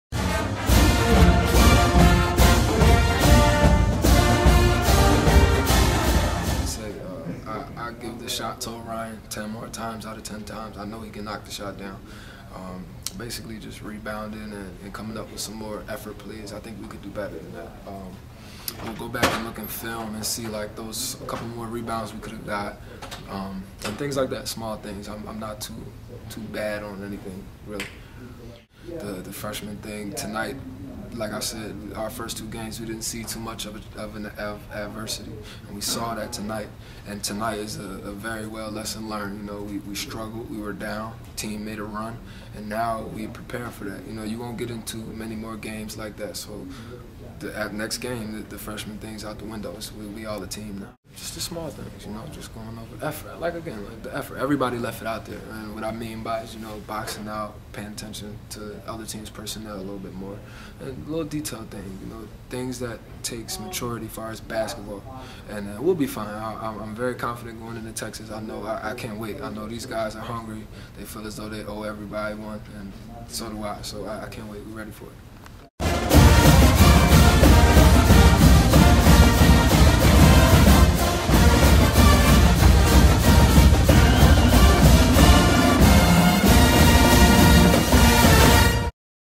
Runnin' Rams: George Mason Post-game
Senior guard Jamal Wilson talks to GoRhody.com following his 38-point performance at George Mason on Nov. 11